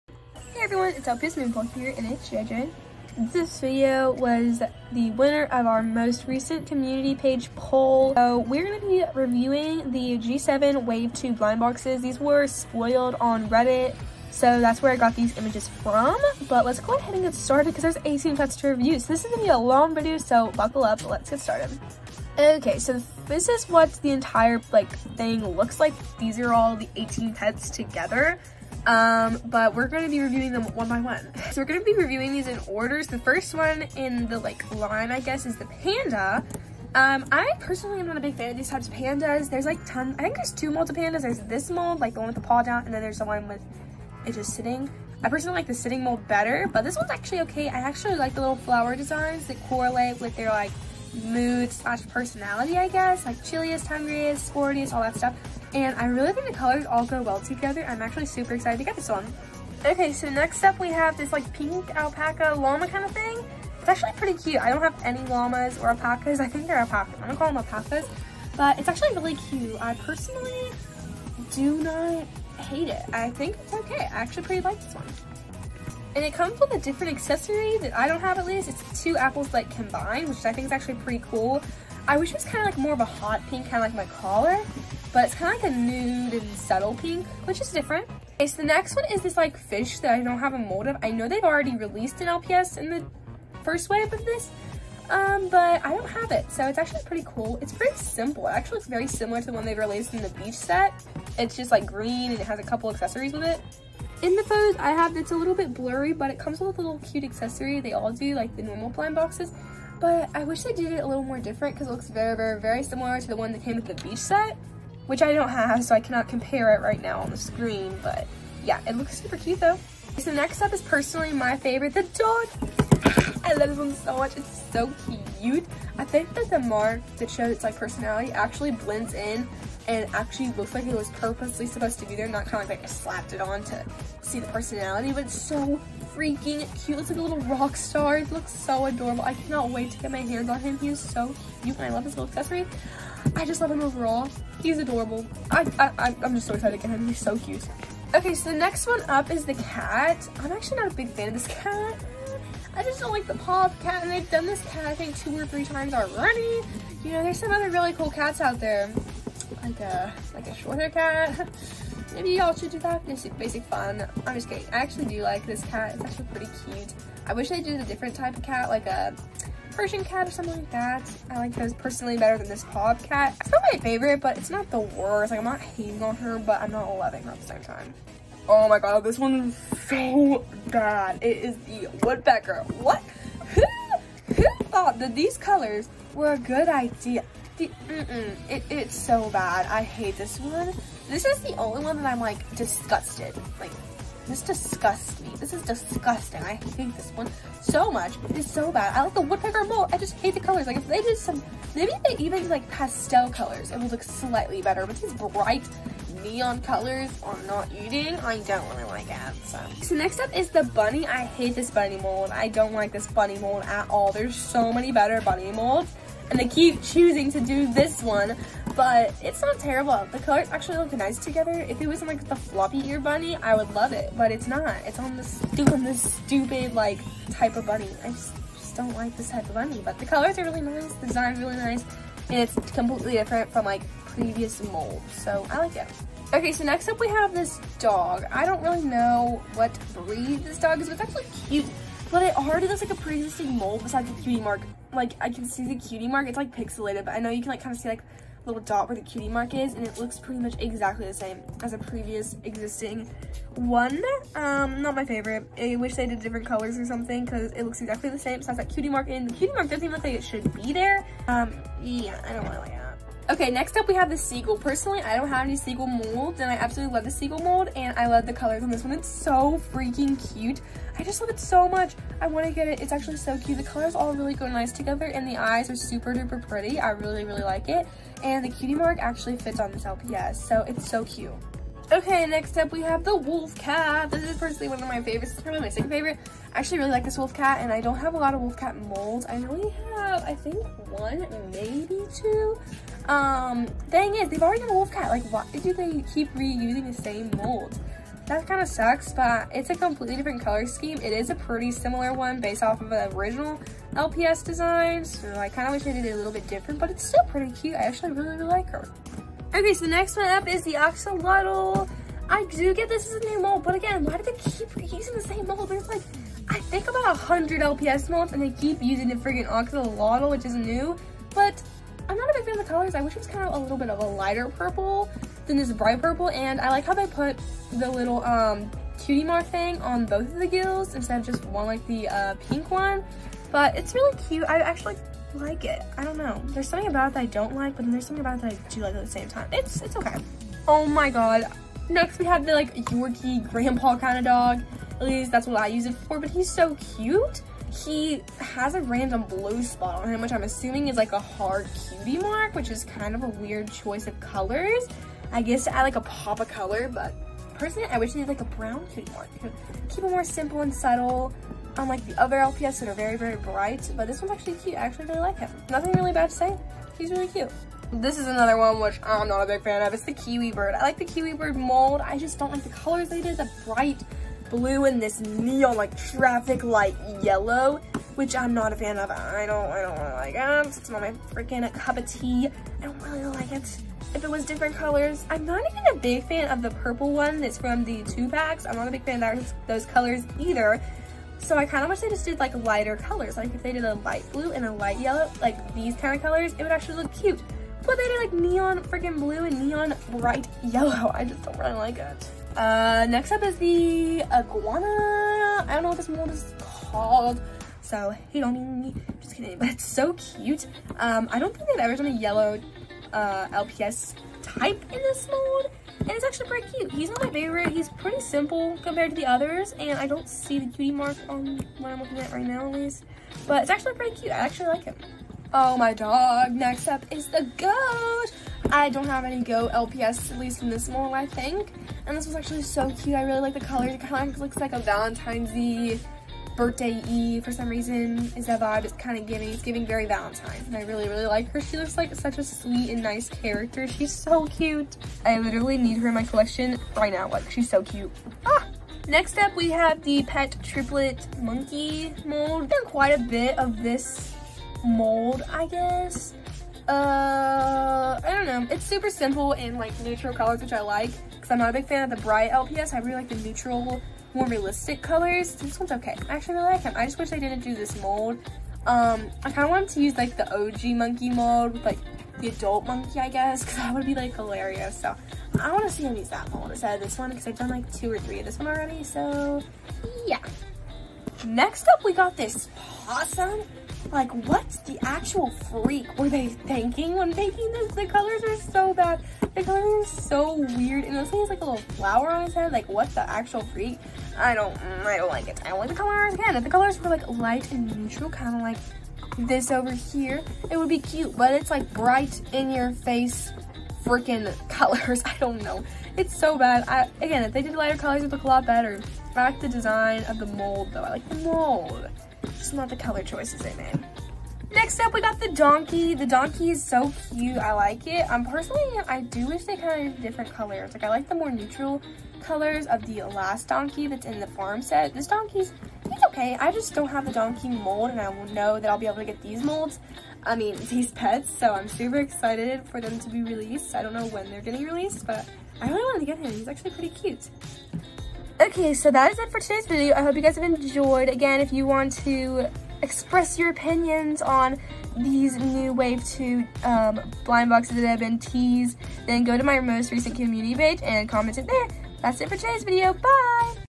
Revewing the wave two lps blind boxes! (G-7)
in today's video I'm going to be review the new wave (wave two) of the G-7 lps blind boxes! I loved all of these pets (but one XD)
Hope you enjoy or enjoyed
I used reedit for the pics!